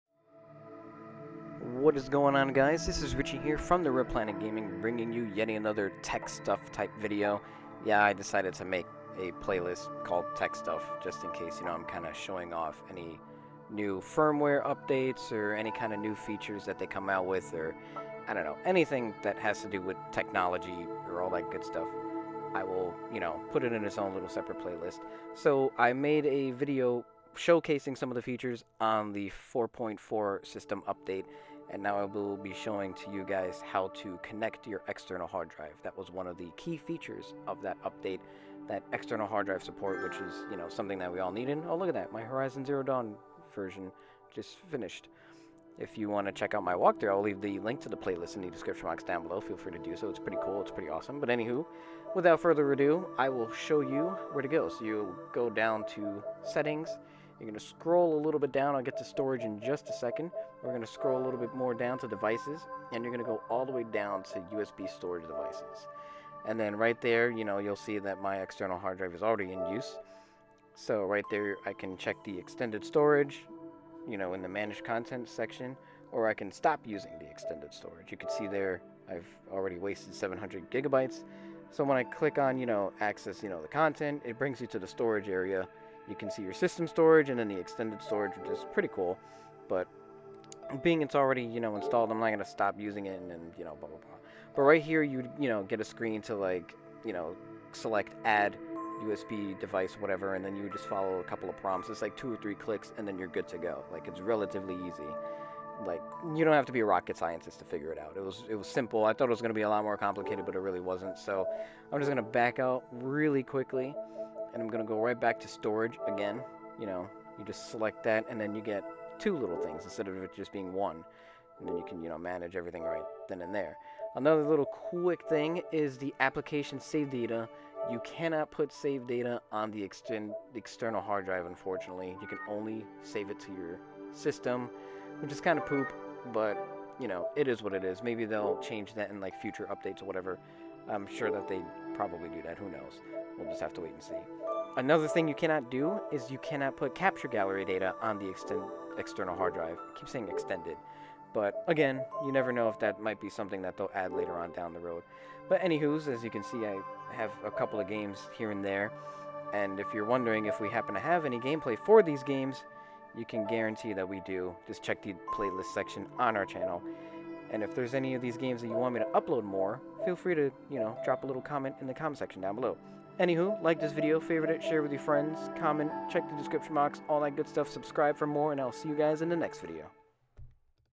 PS4 Firmware 4.50 | How To Install External HDD
With Sony dropping its newest firmware update 4.50, we show you to simply install an external hard drive to add extra storage to your PS4! If you have any questions such as what hard drive I bought, the space and what's the highest amount of storage compatibility, feel free to drop a comment.

Friends & Partners! 🤙🏼

TWITTER: @REDPLANETFILMS, @RichieRich93_ & @chrispopzz

INSTAGRAM: @_popzzz, @redplanetfilms & @picurperfectmoment